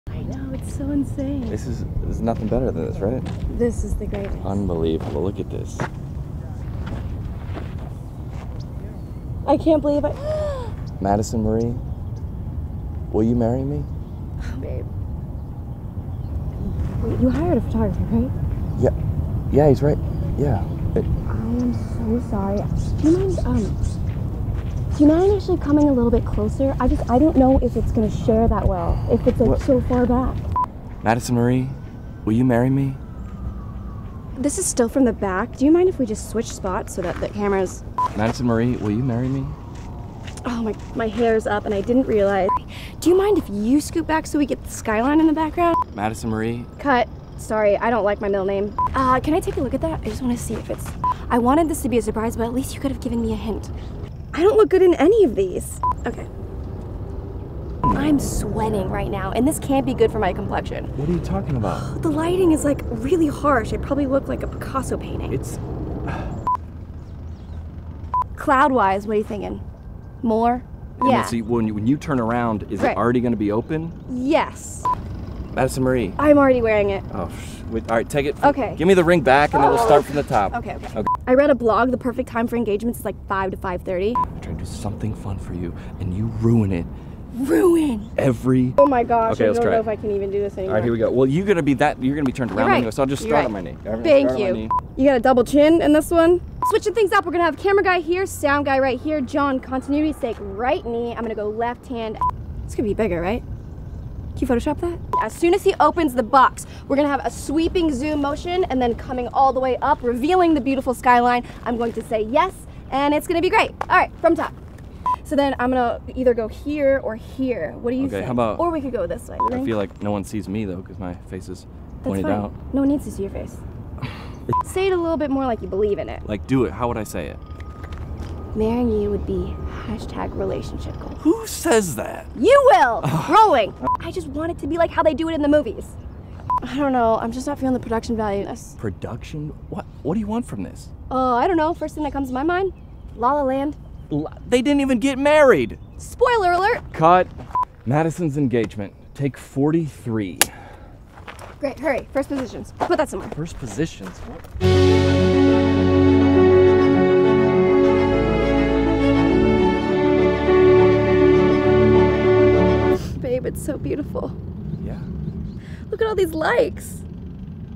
The Millennial Marriage Proposal
The Millenial Marriage Proposal with John Crist and Megan Batoon. Because what's the point of getting engaged if you don't post it on Instagram?

Written, Produced and Directed by John Crist. For more videos, bio and tour schedule,